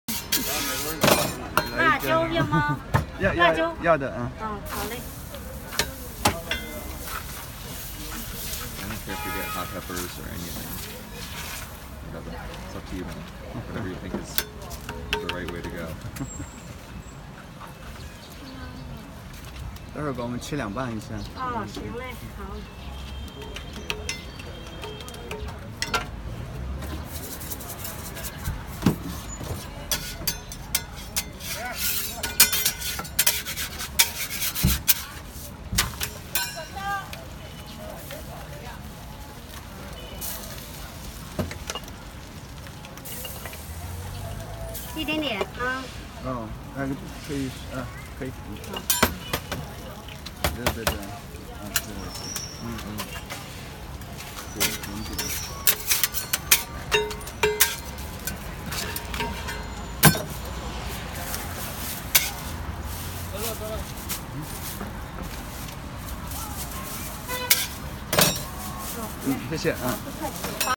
Street food - Jianbing in Beijing China
In the Niu Jie residential district in Bejing China.  This amazing Jianbing cost about 8 RMB, which is about $1.30 USD